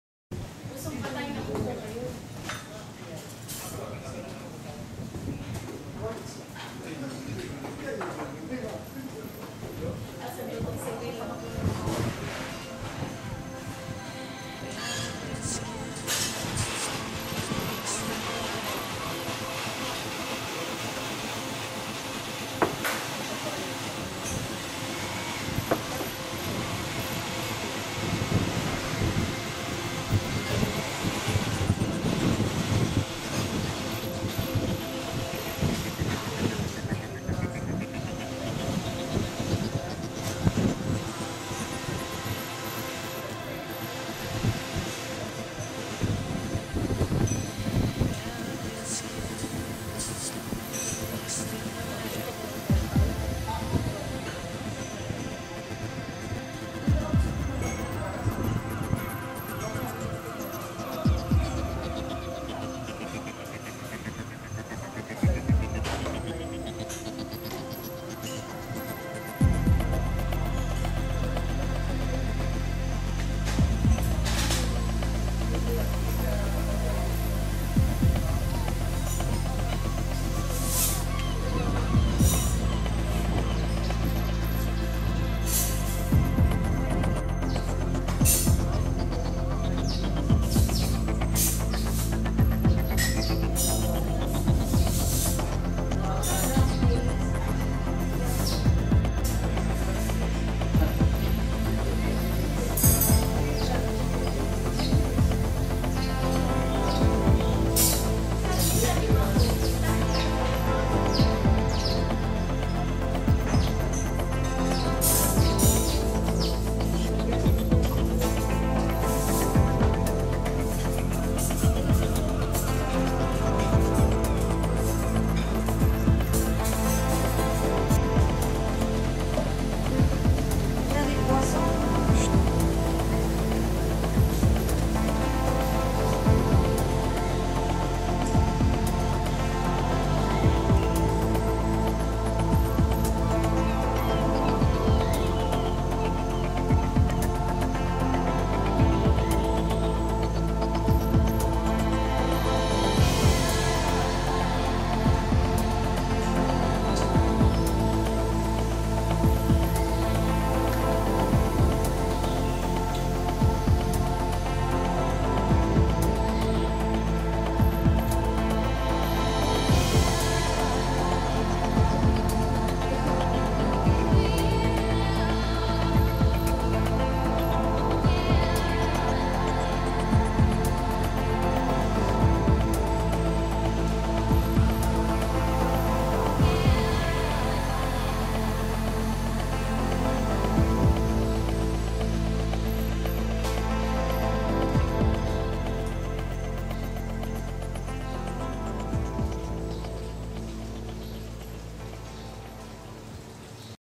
Philippines 2010 - Puerto Princesa, Palawan (Badjao Seefront)
Abad Santos extention, on the mangroove, bar-restaurant on Puerto Princesa bay.